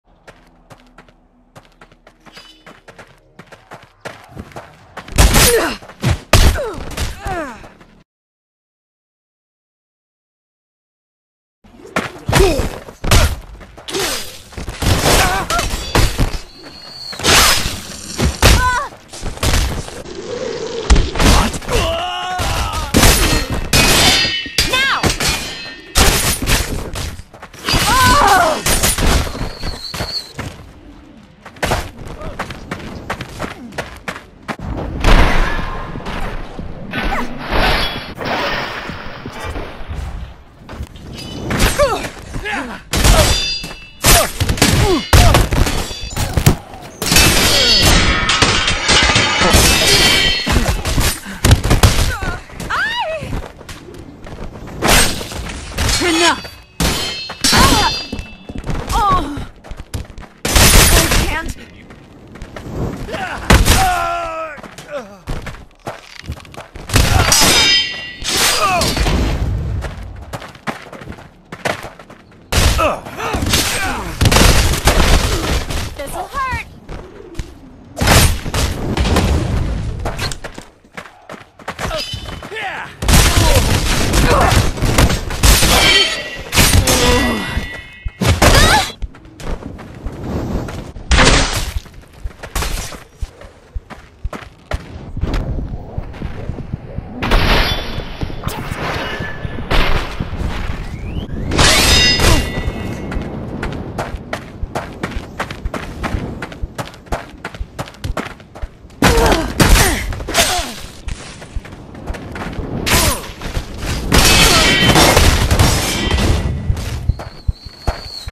Links Going In!
Link shows no mercy in this battle with hardly any slow motion!